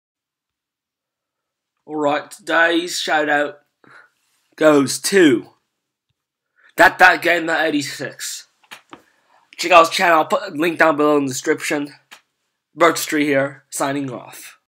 Shoutout to my Fans!
Today I will be shouting out my first person on my youtube channel at 100 subscribers next shoutout will be at 500 subscribers.
Here is the link to his channel